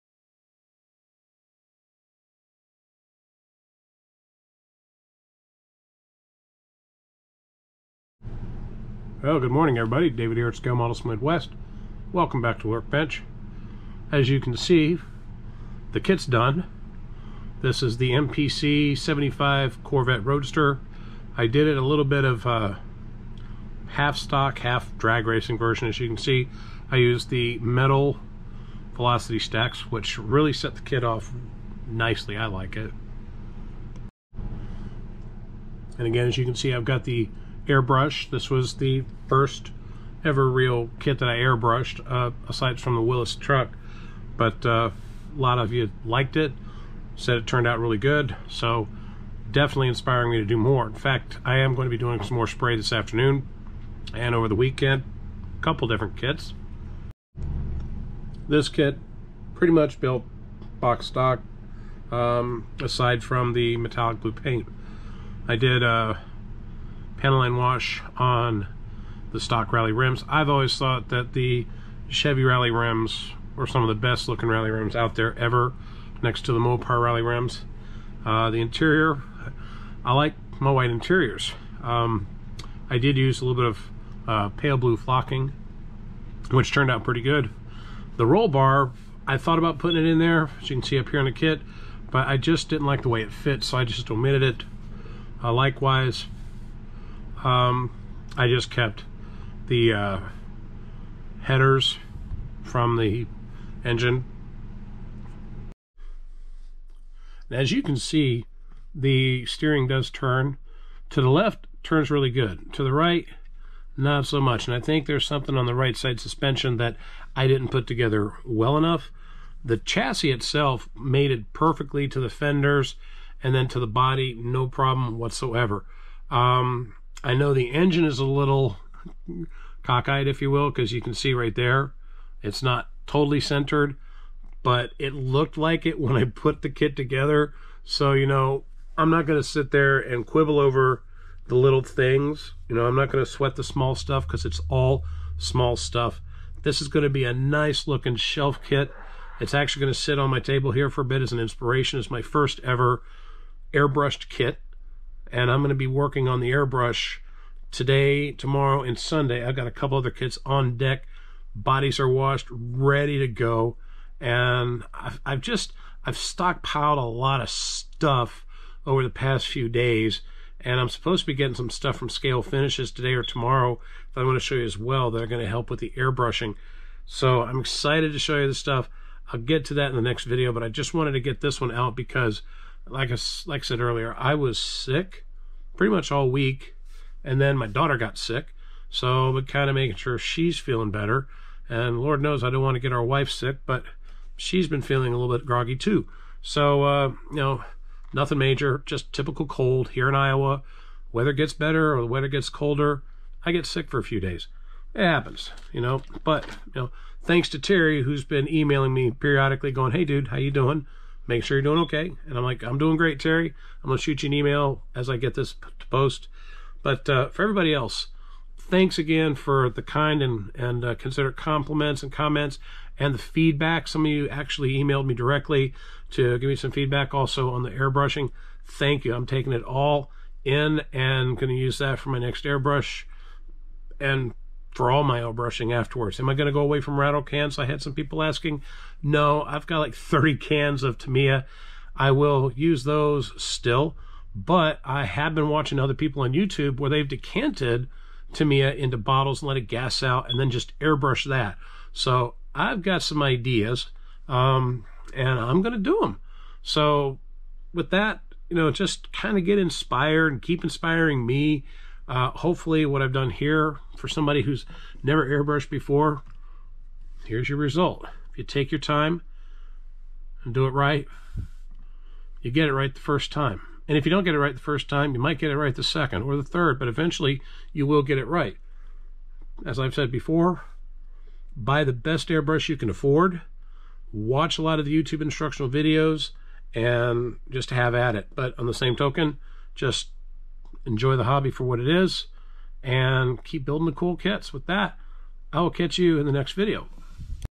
MPC's 1975 Corvette - My FIRST real airbrush, and turned out NICE!
Here's the final of the MPC 1975 Corvette.  This was the kit I won at my first-ever model car show last year, and also my second-ever airbrush attempt, which turned out great.   Again, paint is Tammy's X-13 Metallic Blue, and it's thinned using Mr. Color 400 Leveling Thinner, sprayed through a Grex Genesis airbrush.   If you've been on the fence about airbrushing, use this and my previous video as inspirations to jump off the fence and grab an airbrush of your own and give it a try!

Want to swap shop cards or share pics of your builds for everyone to see?  Thanks!